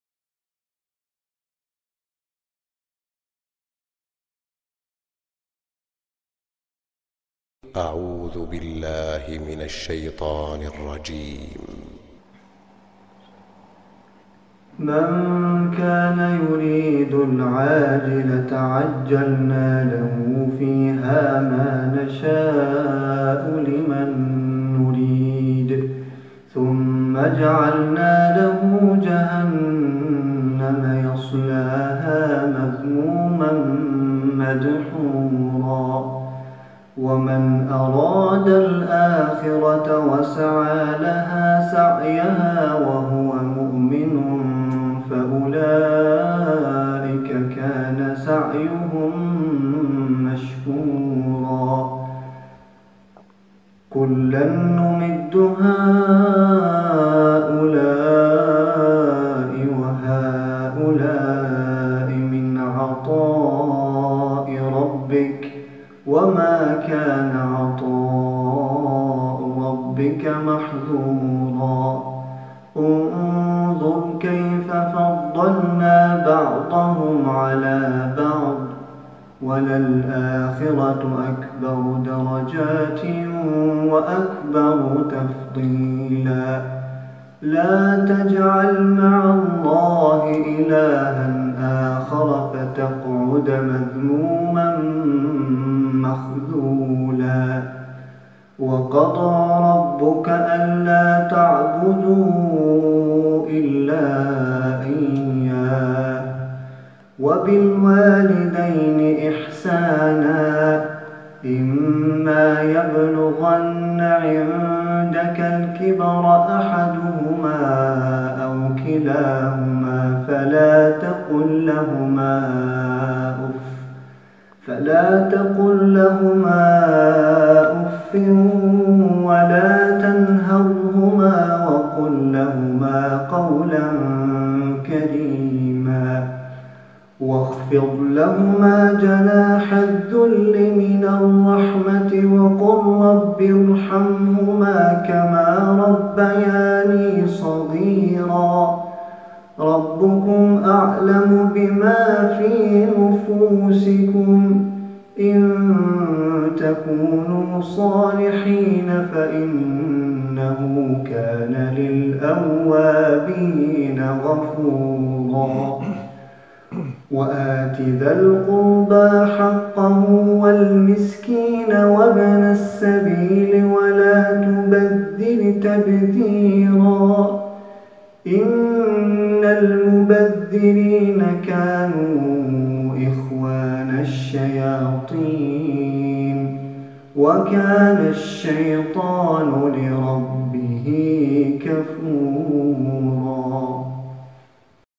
Recitim i butë - Surja EL-ISRÂ’:18-27 - Hfz. Faruk Jashari
Hoxhë Hfz Faruk Jashari, Surja EL-ISRÂ’: 18-27.

Mos na harroni ne lutjet e juaja!